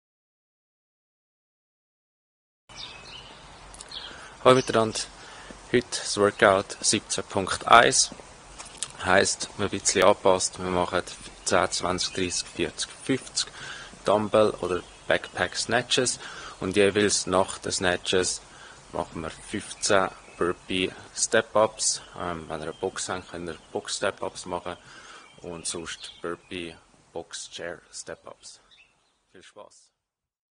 CrossFit Gleis 10 - Home Gym - Day13 - (29.03.2020)
17.1 Home Gym Version
10 BP Snatches
15 Burpee Chair Step Ups
20 BP Snatches
15 Burpee Chair Step Ups
30 BP Snatches
15 Burpee Chair Step Ups
40 BP Snatches
15 Burpee Chair Step Ups
50 BP Snatches
15 Burpee Chair Step Ups